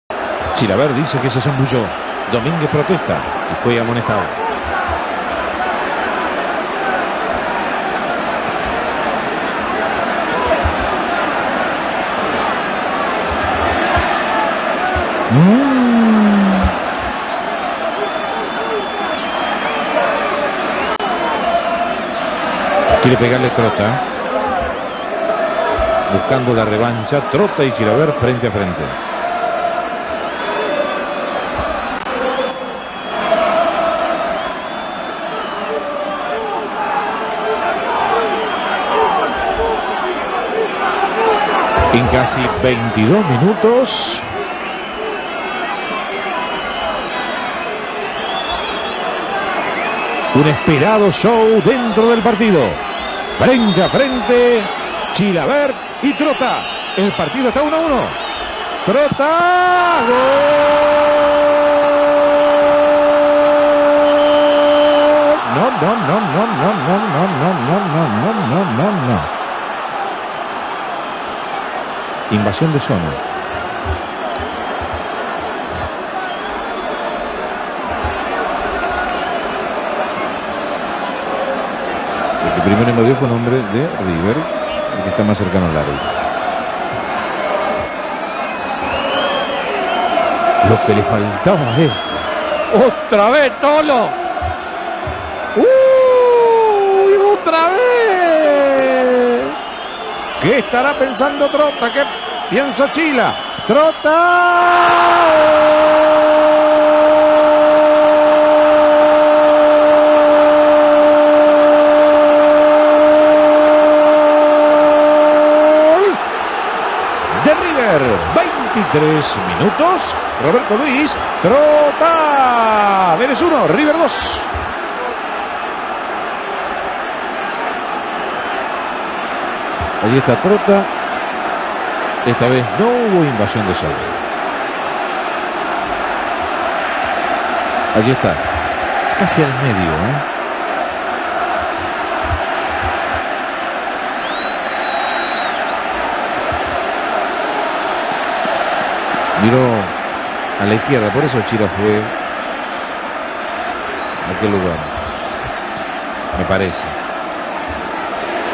Dos penales: Trotta sorprende a Chilavert (y a Vélez)
Roberto Trotta y José Luis Chilavert fueron compañeros en Vélez Sarsfield y fueron campeones del mundo. Eso no le impidió a Trotta, cuando fue a River, criticar a la hinchada de Vélez que esperaron el momento de esperarlo en Liniers para insultarlo. Chila no se quedó atrás y dijo cuando le consultaron sobre Trotta que "con cornudos no habla". Esto ocurrió en el Torneo Clausura 2000 de Argentina.